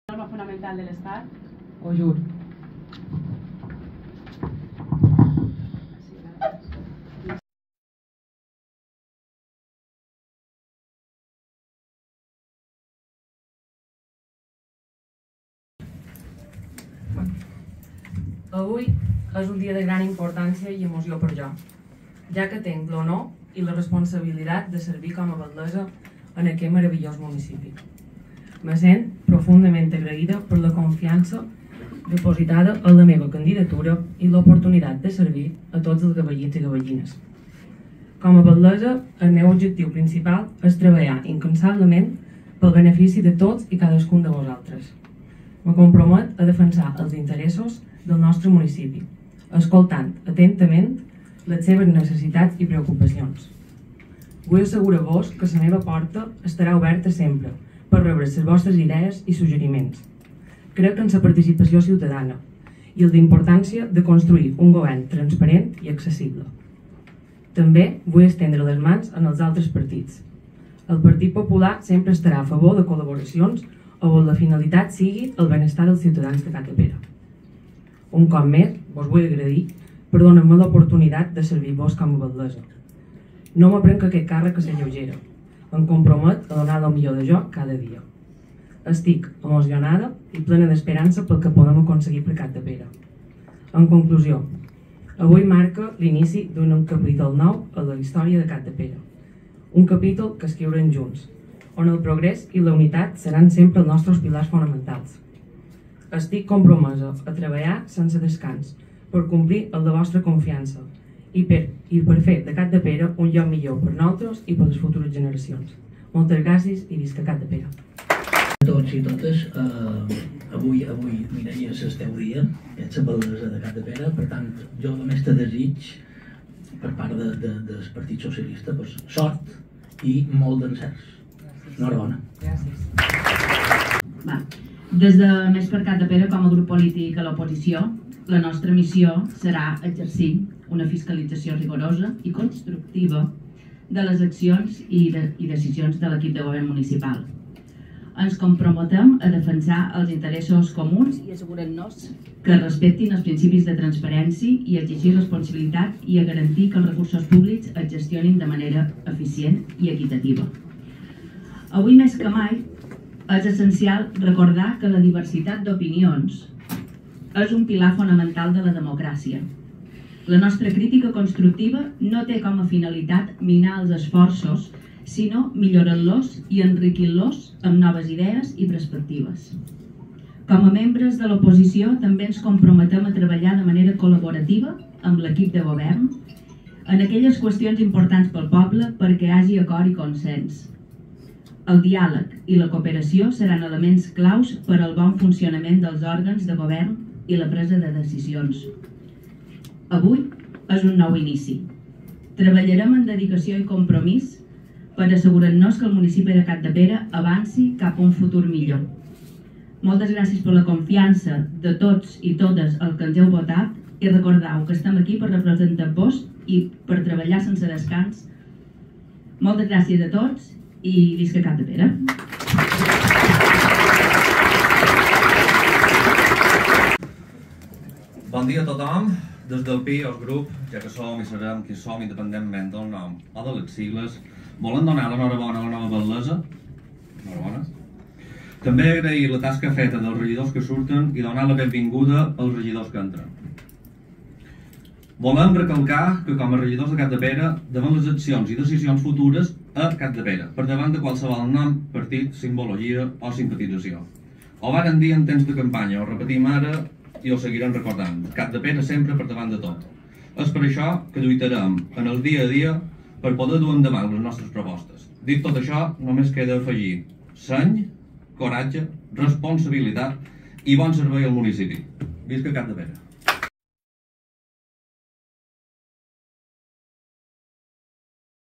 Acte de constitució de l'Ajuntament de Capdepera 2023-27
Jurament de la batlessa Mireia Ferrer i intervencions de tots els portaveus dels partits polítics.